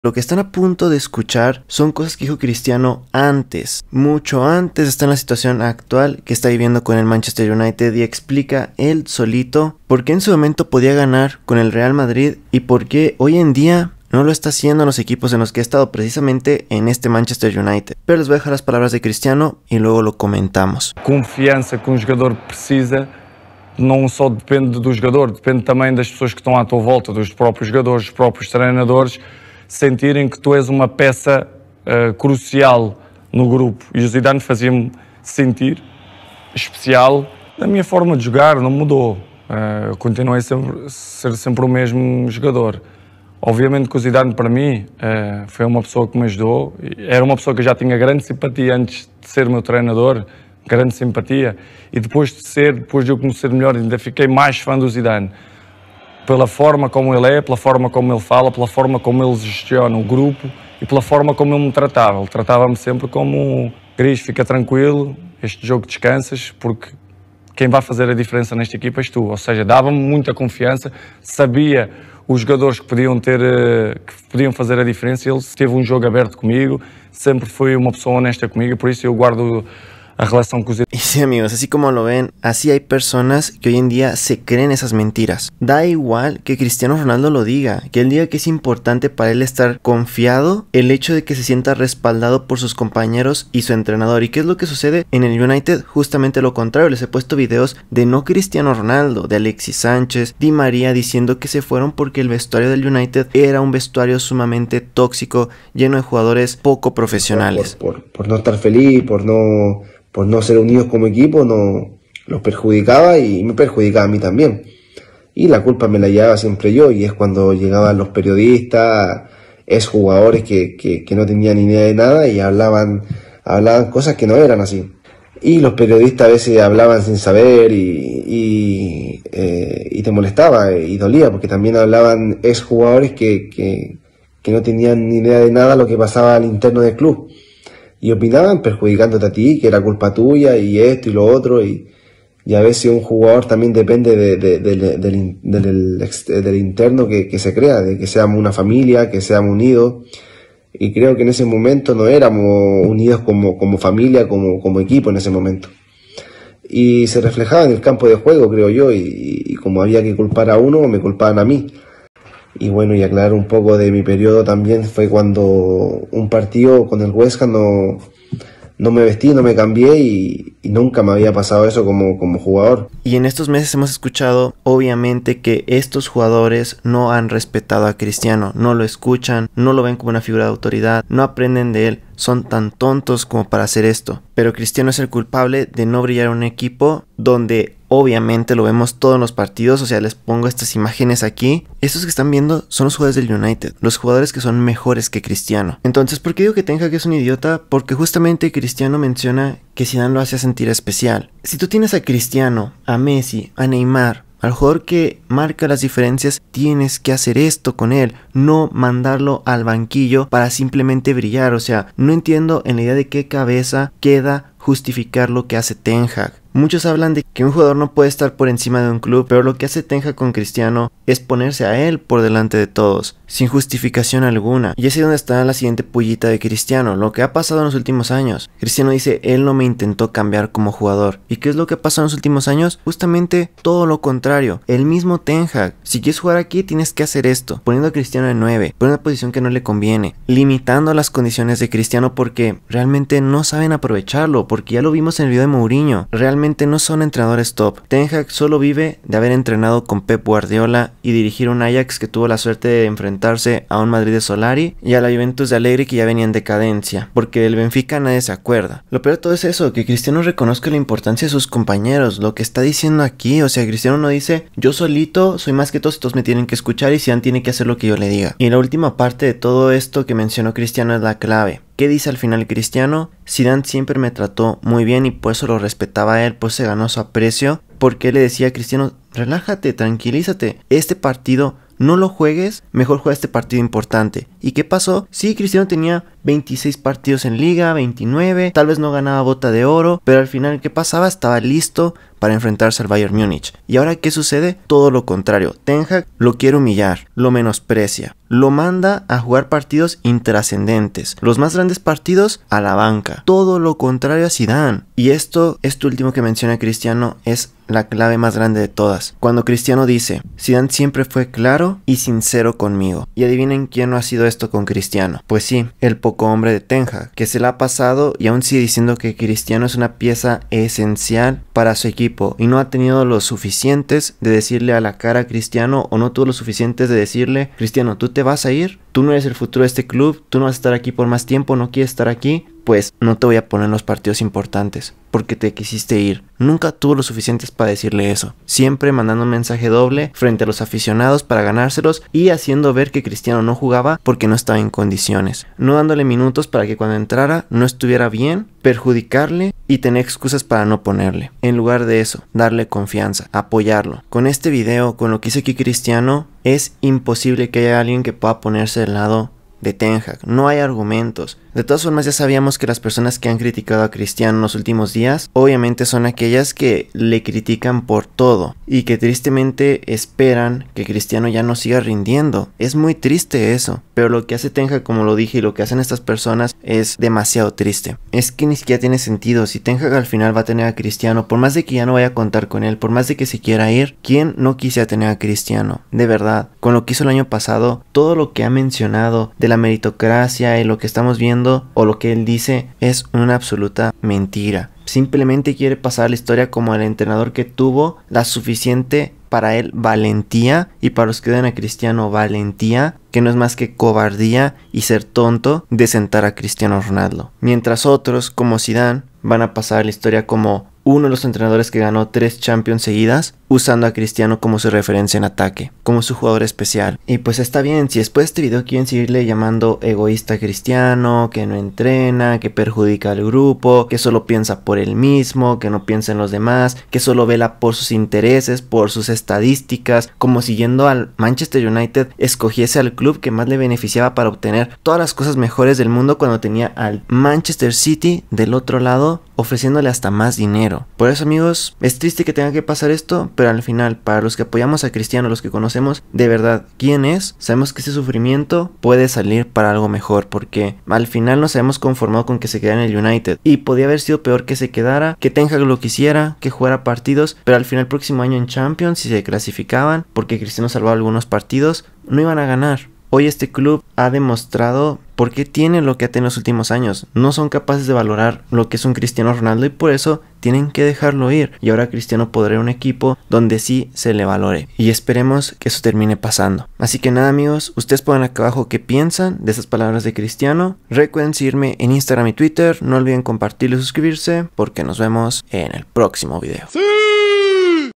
Cristiano Ronaldo EXPLICA porque GANABA con ZIDANE y por qué TEN HAG NUNCA PODRÁ SER COMO ÉL
Cristiano Ronaldo EXPLICA porque GANABA con ZIDANE y porque TEN HAG NUNCA SERÁ IGUAL / MOU ACABA CON LAS MENTIRAS DE TEN HAG Y EL MANCHESTER UNITED, CR7 DEBE IRSE, ANCELOTTI, ZIDANE y FERGUSON  verdadero DTs